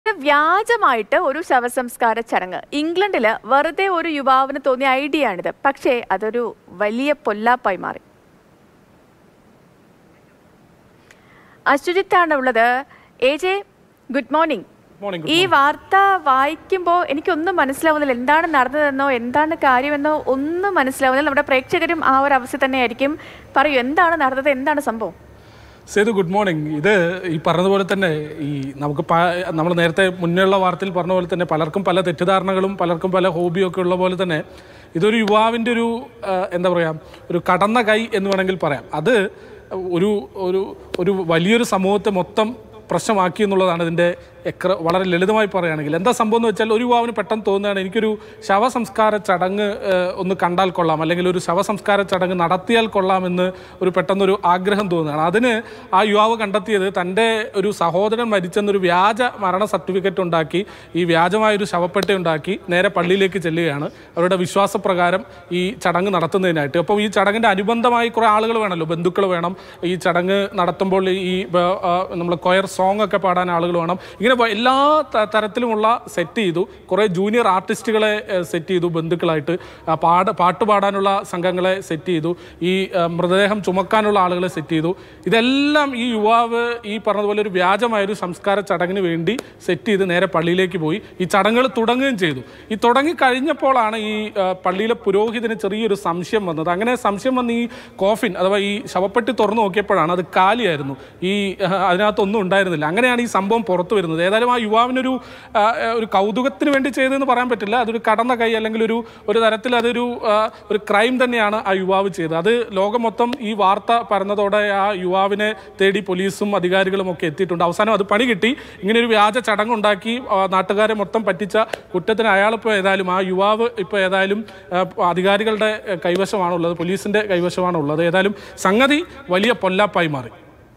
വ്യാജമായി ഒരു ശവസംസ്കാരചടങ്ങ്; യുവാവിന് വെറുതെ തോന്നിയത് പക്ഷെ പൊല്ലാപ്പായി മാറി
വ്യാജ മരണസർട്ടിഫിക്കറ്റും ശവപ്പെട്ടിയും വ്യാജമായി ഒരു ശവസംസ്കാരചടങ്ങും; യുവാവിന് വെറുതെ തോന്നിയത് പക്ഷെ പൊല്ലാപ്പായി മാറി

Happy viewing!